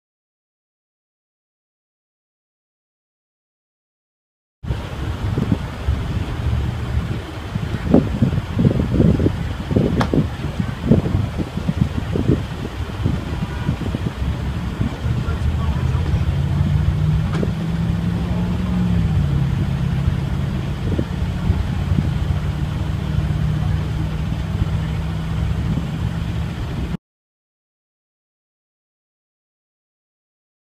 See flames shoot across Dort Highway following gas leak explosion in Flint
Flint police shutdown Dort Highway between Court Street and Lapeer Road following a gas leak explosion Saturday, June 6. The flames could be seen shooting over nearby Interstate 69.